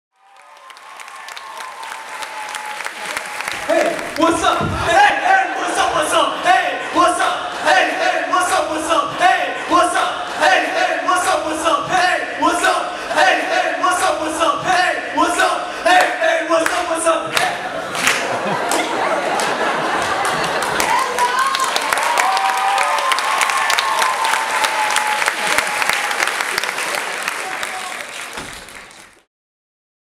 Variety Show 2015!
Albany High School
8th Annual Variety Shows, May 28 and 29
Thanks to Alex Caceres for the plug!